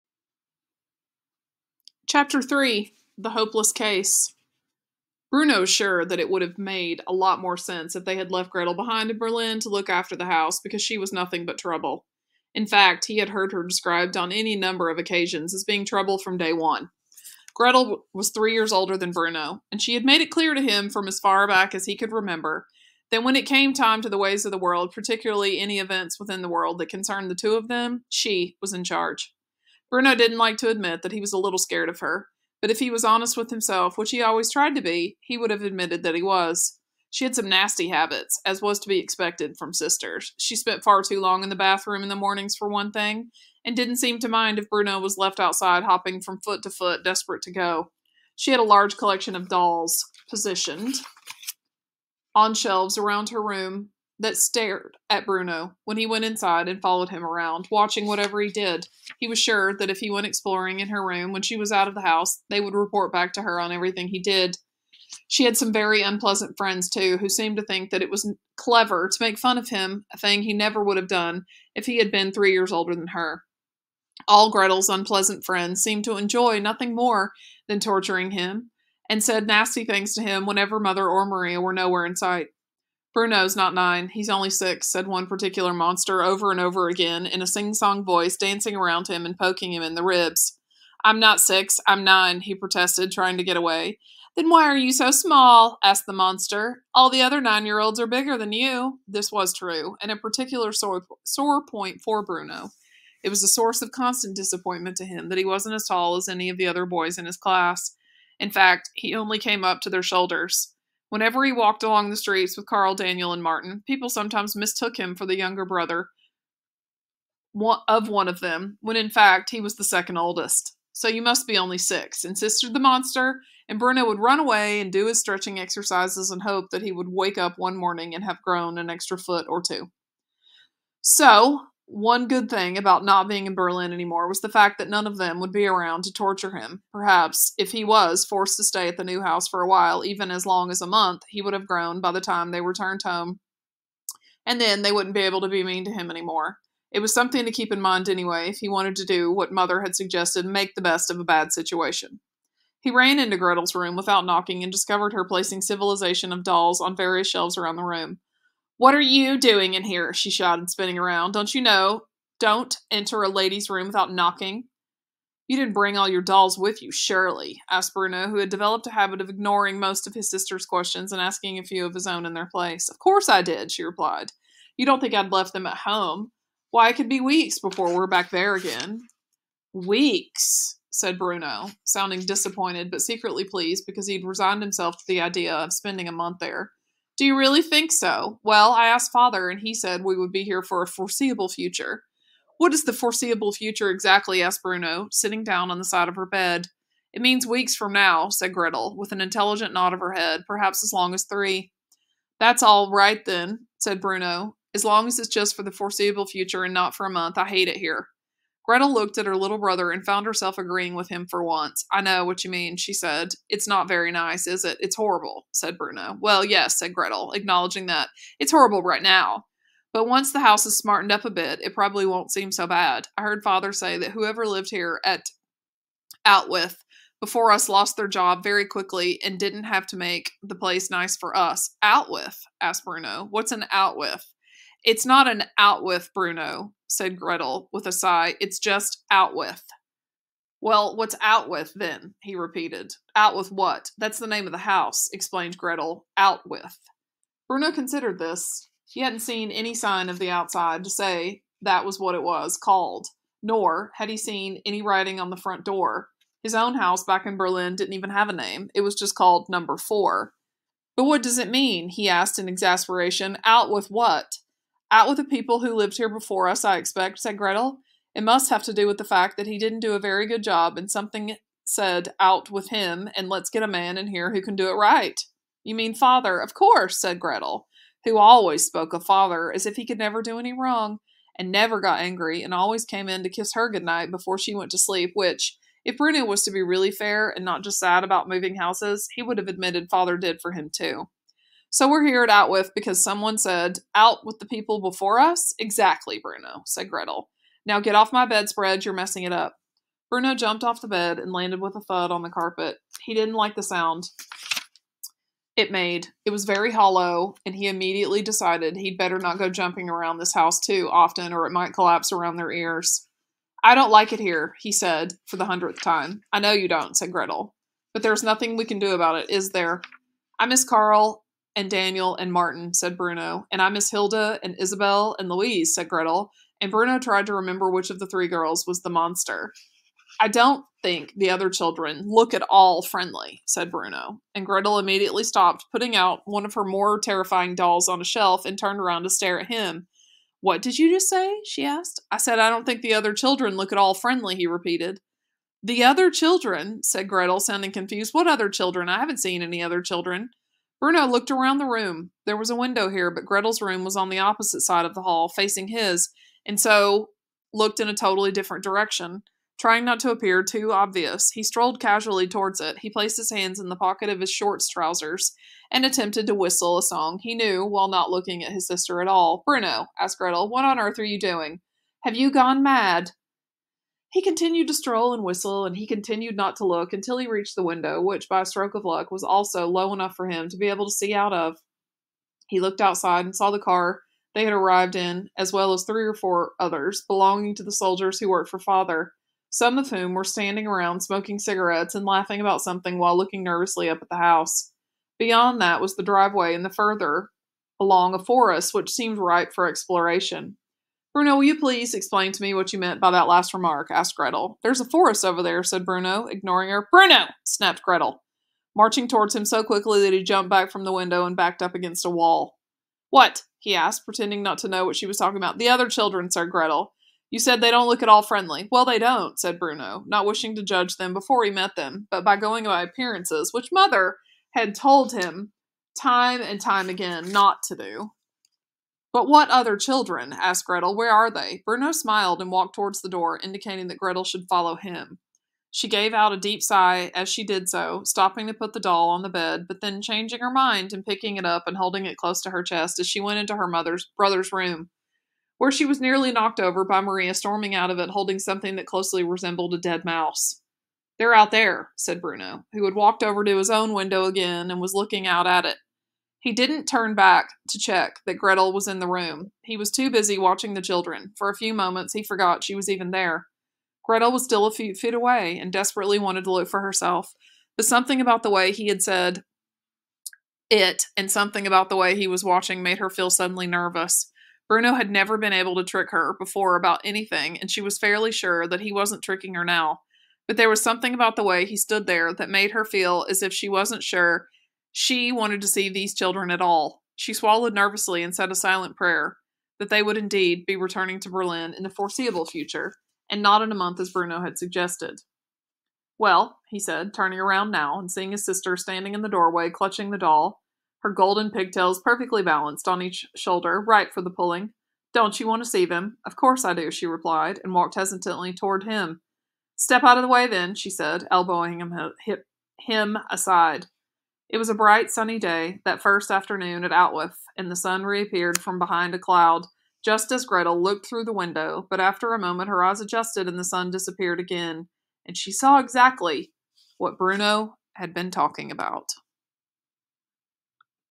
The Boy in the Striped Pajamas Chapter 3: The Hopeless Case
Teacher Read Aloud, Audiobook